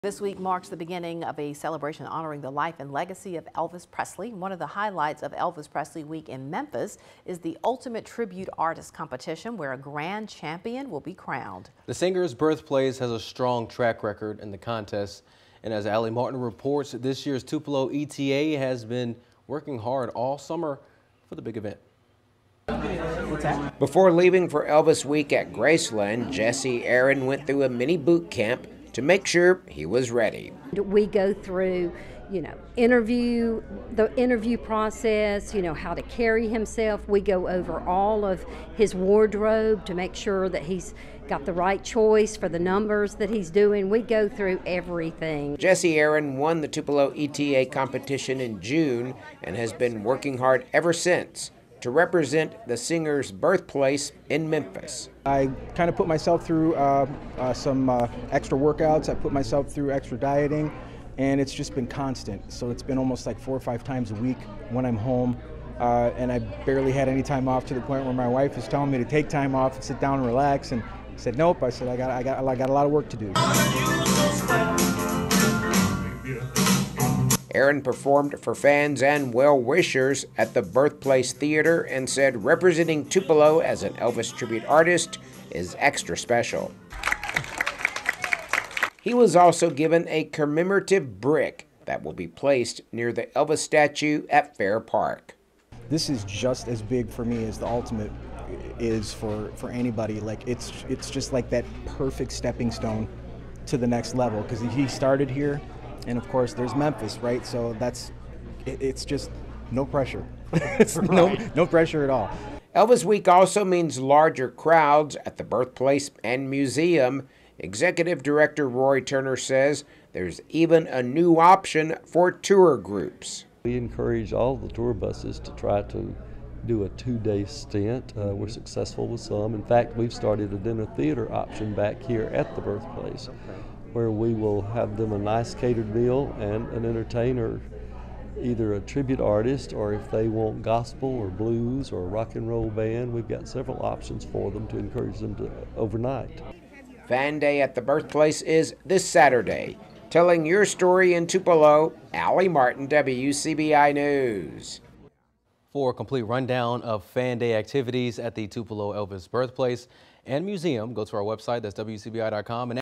Tupelo’s Ultimate Elvis Tribute Artist champ sets sights on Memphis
The winner of Tupelo's Ultimate Elvis Tribute Artist competition in June is on his way to Memphis where he will compete for the Grand Prize.